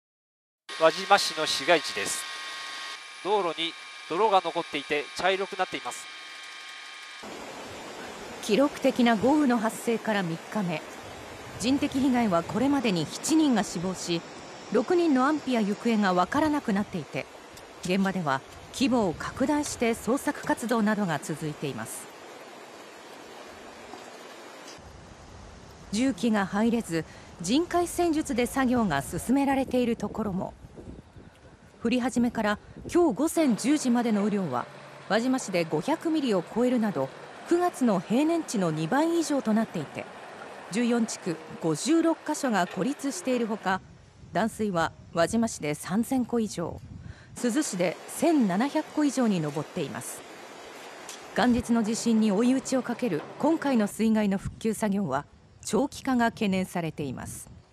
輪島市の水害 復旧作業は長期化か…7人が死亡 6人の安否や行方が不明 重機入れず人海戦術での捜索活動も
石川県輪島市では、規模を拡大して捜索活動などが続いています。（荘司和也カメラマン）
「輪島市の市街地上空です。道路に泥が残っていて、茶色くなっています」

記録的な豪雨の発生から3日目。人的被害は、これまでに7人が死亡し、6人の安否や行方がわからなくなっていて、現場では規模を拡大して捜索活動などが続いています。重機が入れず、人海戦術で作業が進められているところも。

降り始めから、23日午前10時までの雨量は、輪島市で500ミリを超えるなど、9月の平年値の2倍以上となっていて、14地区56か所が孤立している他、断水は輪島市で3,000戸以上。珠洲市で1,700戸以上に上っています。元日の地震に追い打ちをかける今回の水害の復旧作業は、長期化が懸念されています。